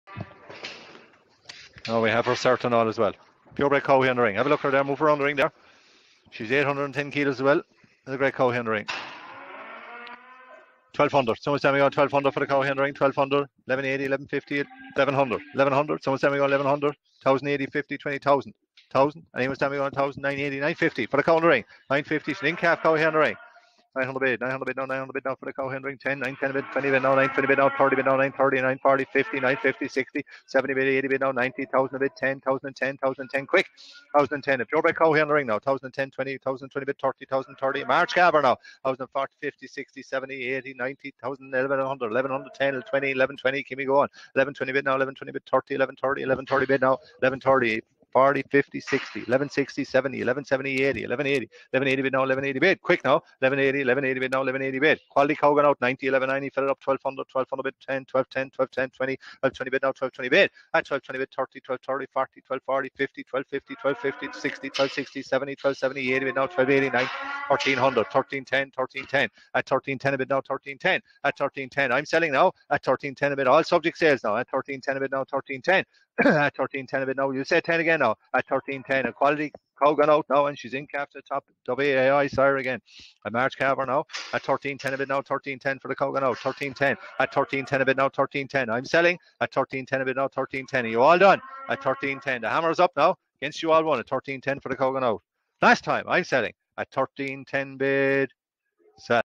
Newport Lot 330 14-01-2021
Highest selling price in Newport this afternoon. Purebred Cow in Lot 330 sold for 1310.